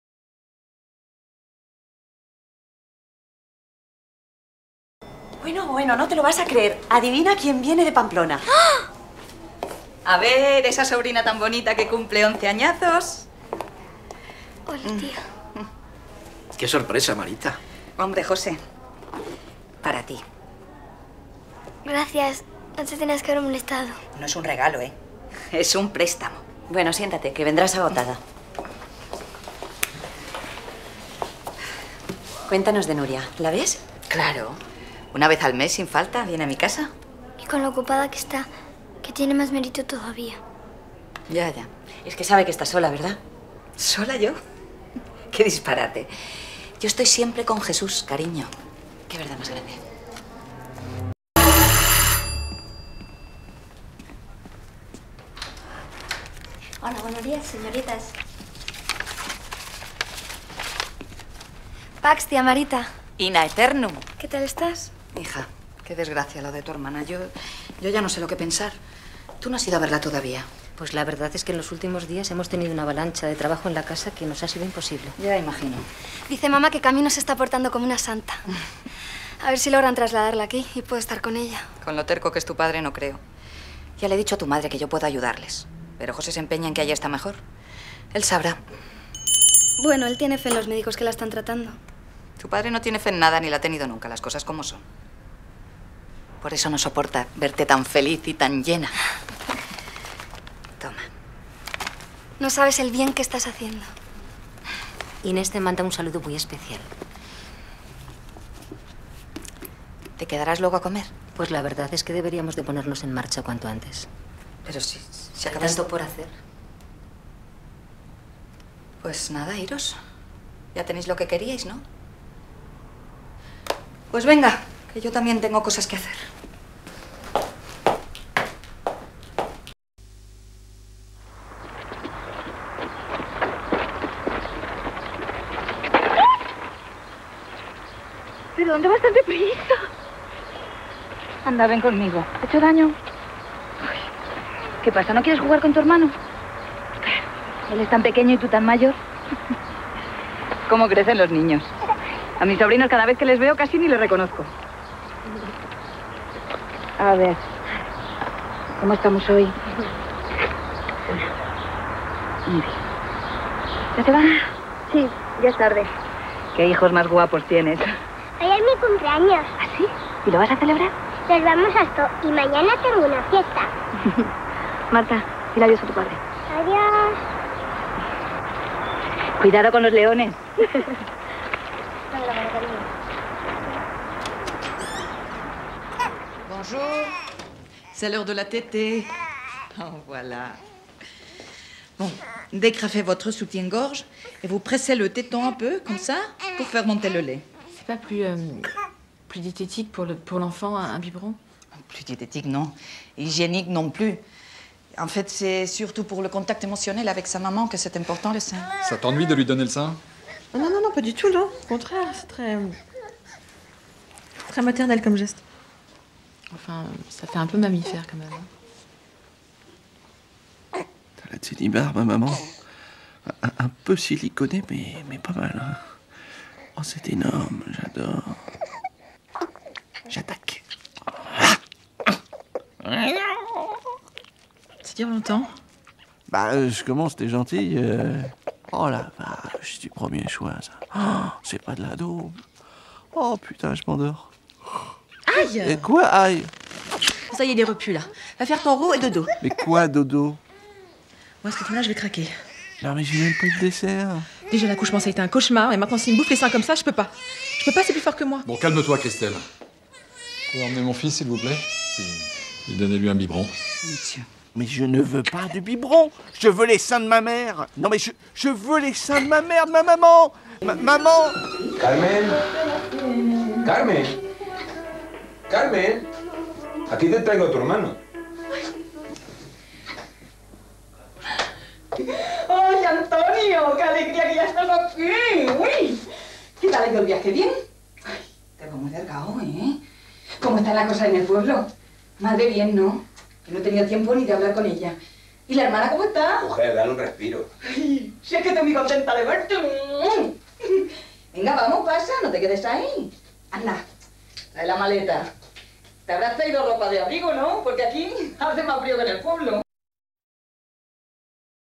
Lola Casamayor - CINE
Lola Casamayor - Videobook con algunas escenas de Lola Casamayor en CINE